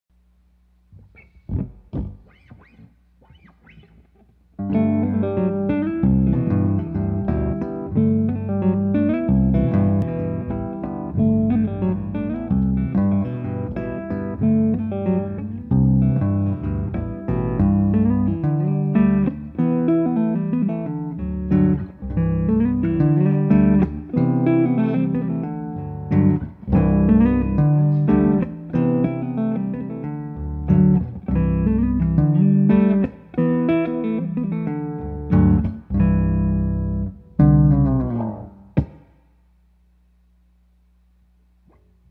Music Update no.whatever
I was at home playing bass VI and Math Rock happened.